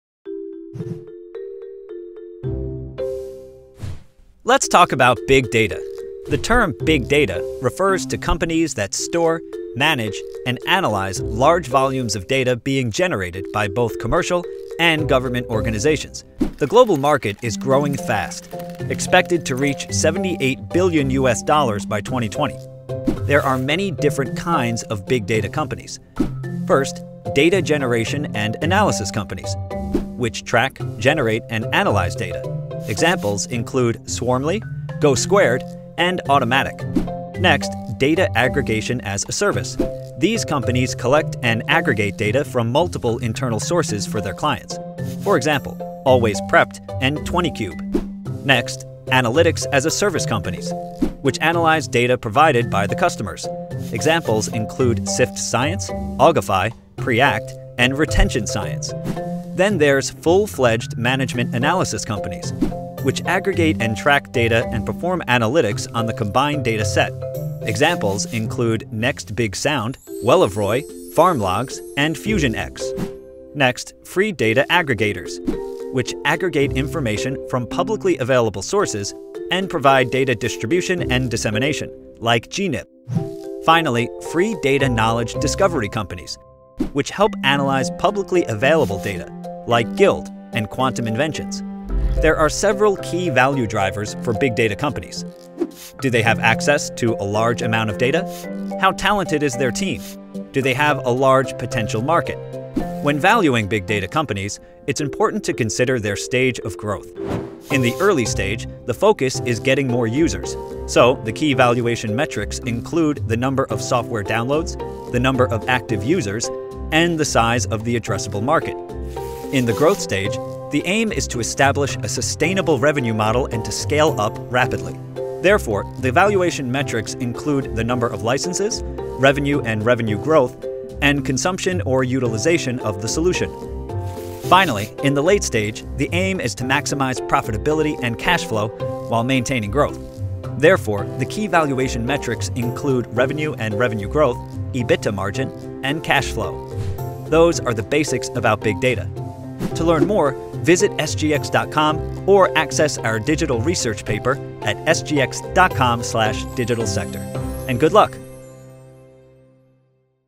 Your Guide to Investing in Big Data Business
Big Data is touted as an area of tremendous growth with predictive analysis and data aggregation as key services that sieve through complex information. Through this video, learn more about what investors should look out for when investing in Big Data companies, particularly the various business models and growth horizon in the digital industry.